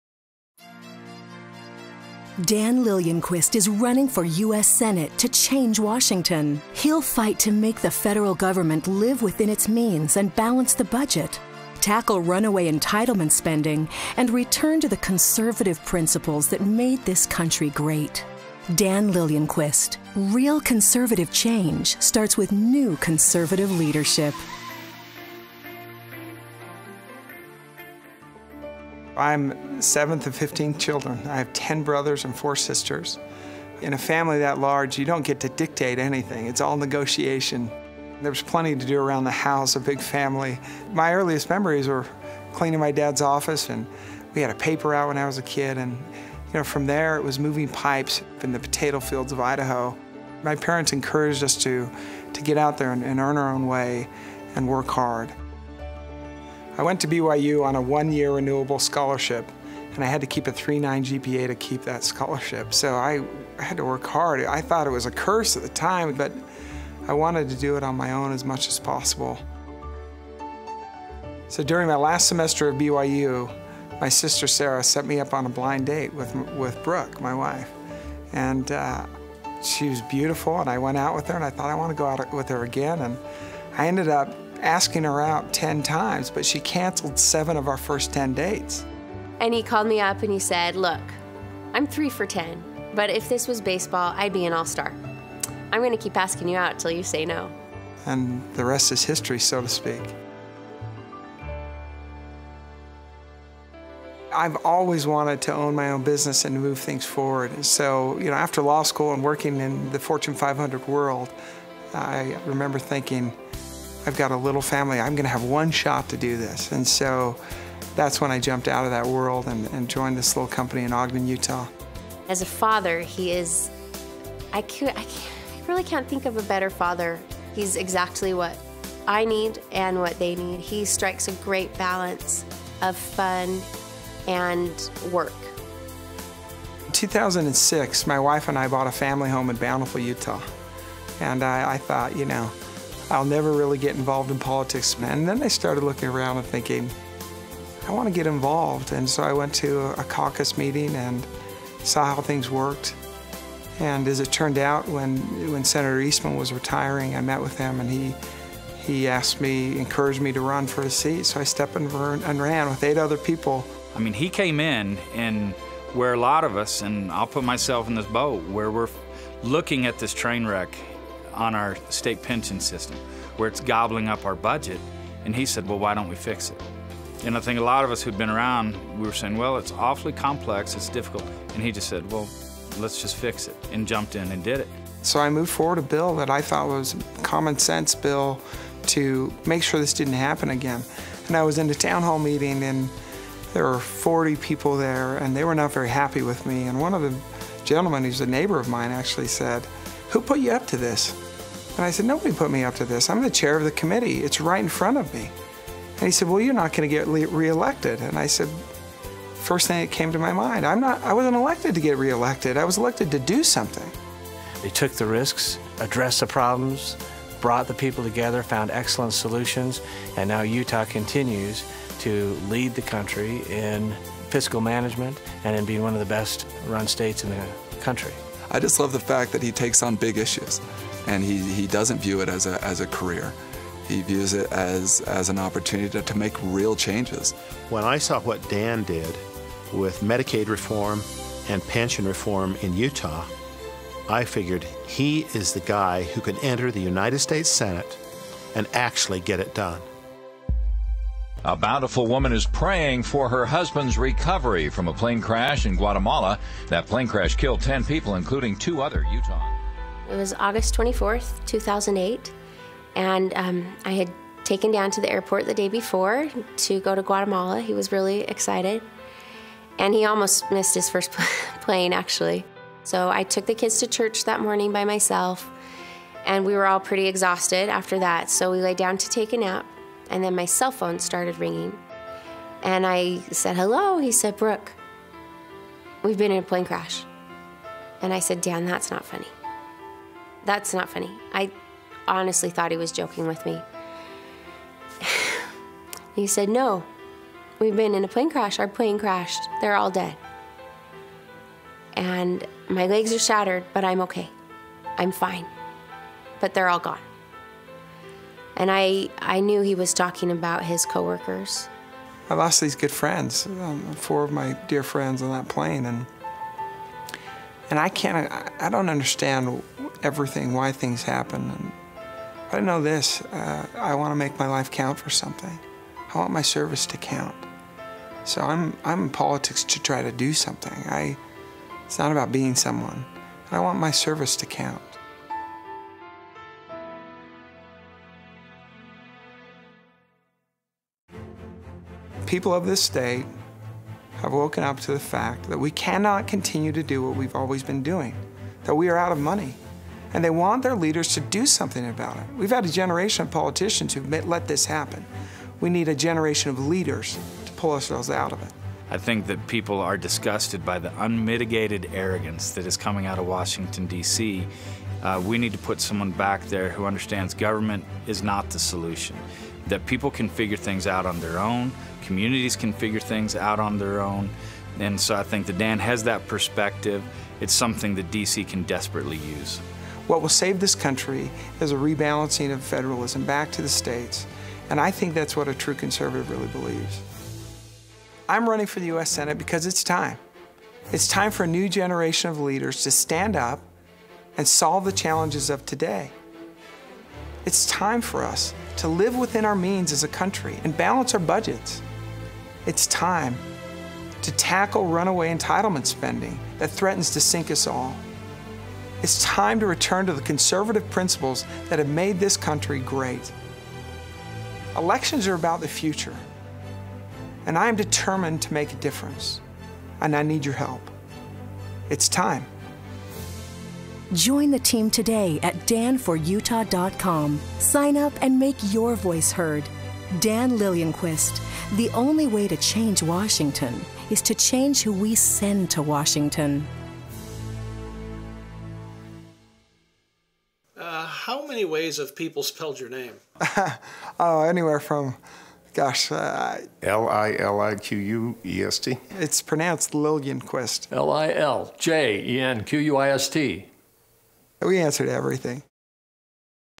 Meet Dan Liljenquist
Take a few minutes to learn about Dan Liljenquist as he enters the race to become the next US Senator representing Utah. It's time!

Dan Liljenquist was named National Legislator of the Year by Governing Magazine. He's a Republican candidate for U.S. Senate in Utah.